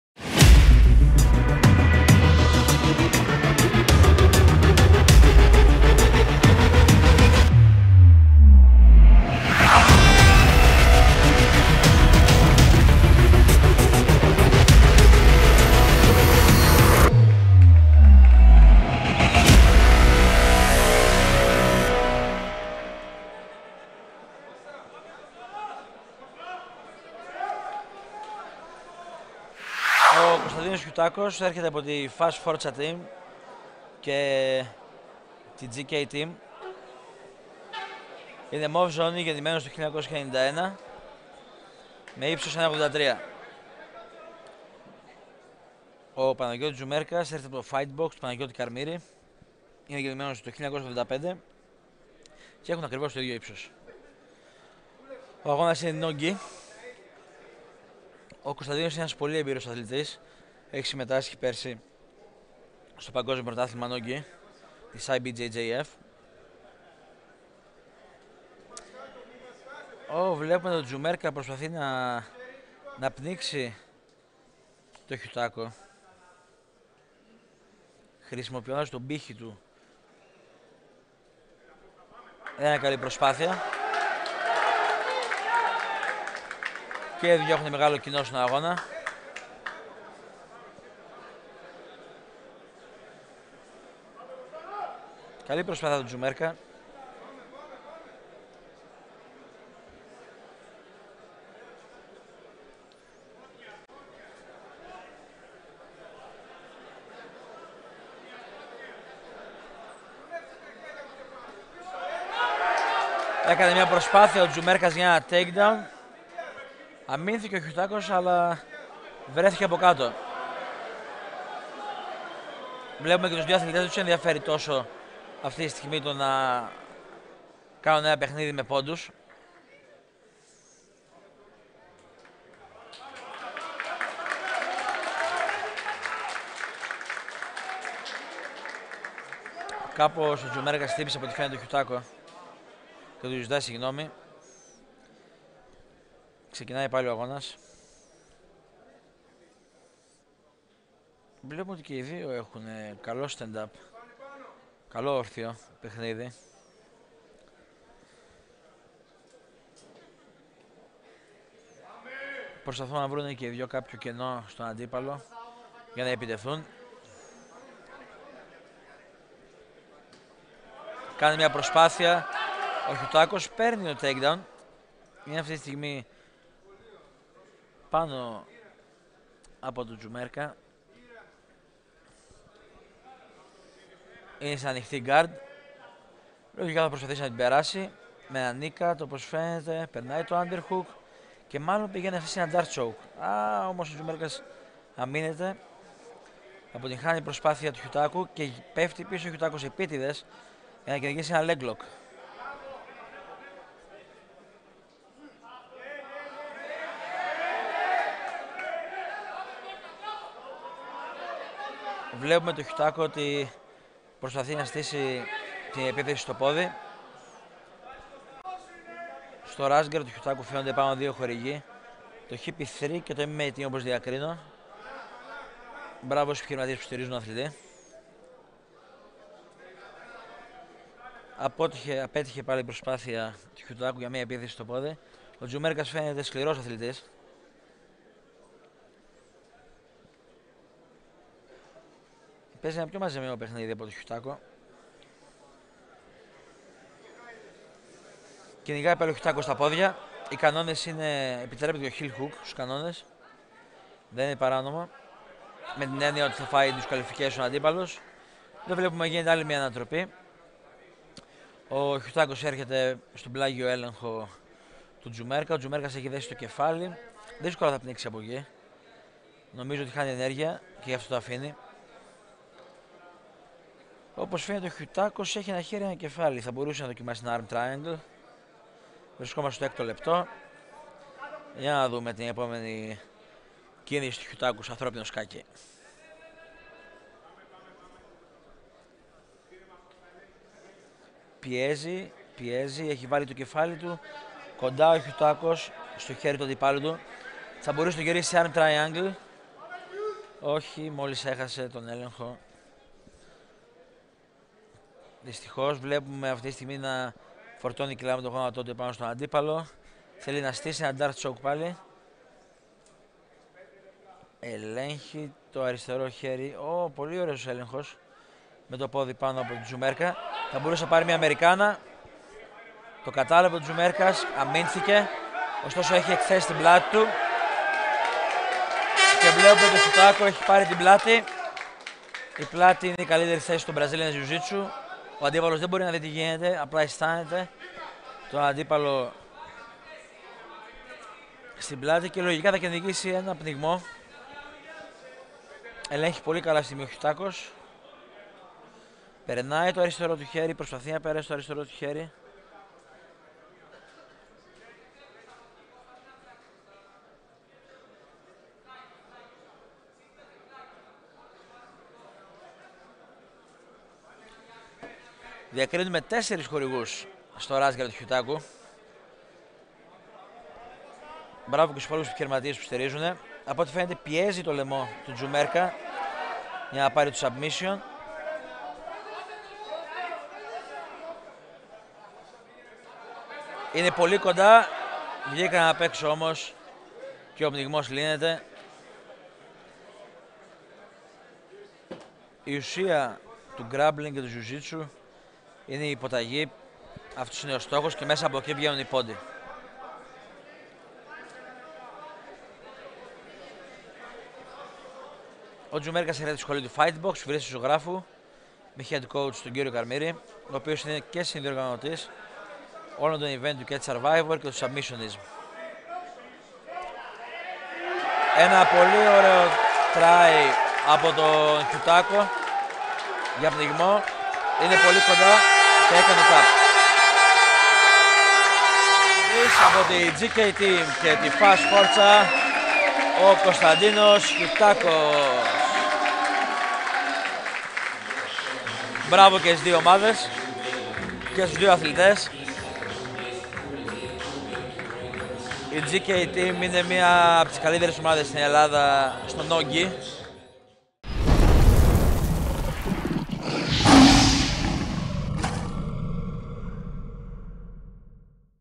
Submissionism 4: Konstantinos Chioutakos vs. Panagiotis Tzoumerkas Full Fight
Submissionism #4 | Sunday, 22th December 2023 | Athens, Greece
Konstantinos Chioutakos (0-0-0, Greece) vs. Panagiotis Tzoumerkas (0-0-0, Greece)

⏱ TIME STAMPS
0:00  Intro
0:09  1st Round
07:09 Winner is.
07:30 Outro

COME SAY HELLO! 👋